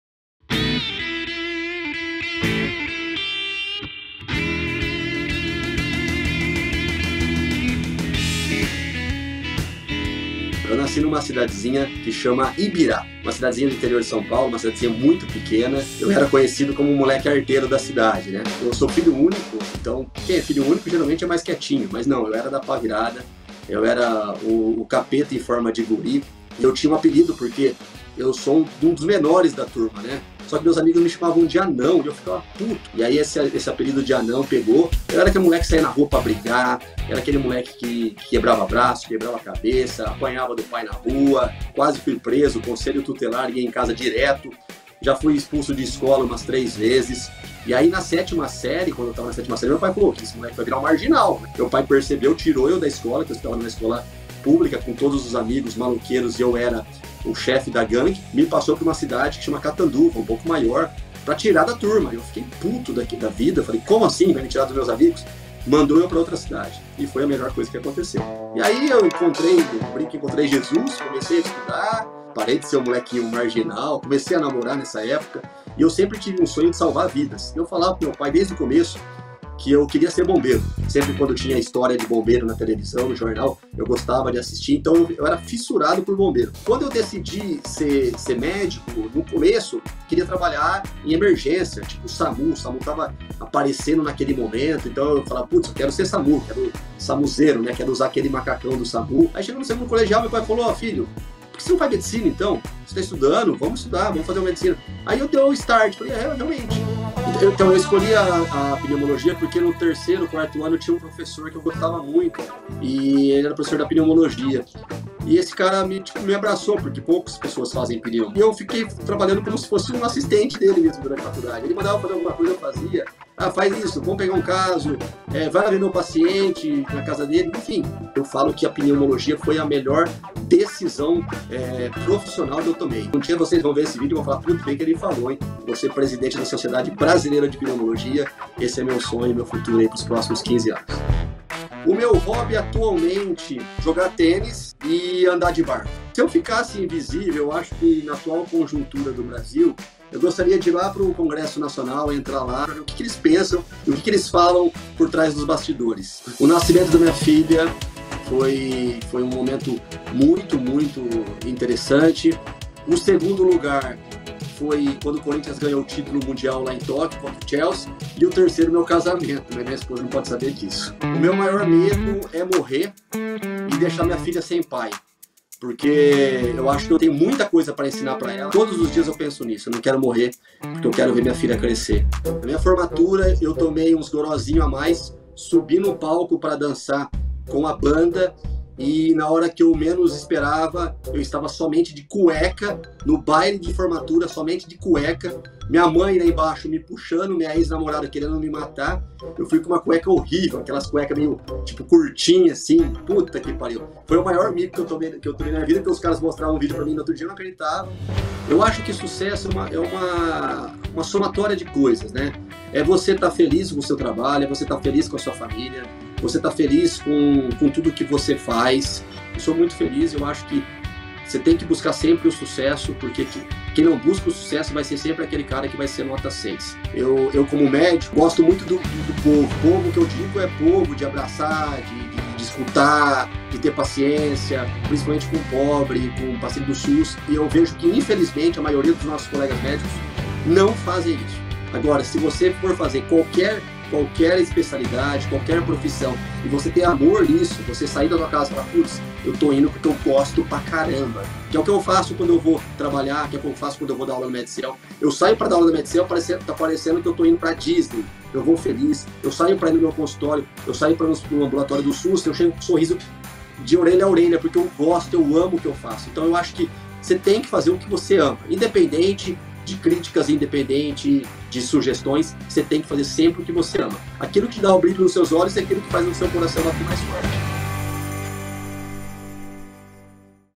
Conheça seu professor Medcel: José Megda
Está preparado para conhecer seu professor Medcel de um jeito que você nunca viu?

Aperte o play e descubra o maior mico, o maior medo e as histórias inspiradoras do José Megda, da Pneumologista.

Episódios sempre às segundas e quartas, às 19 horas!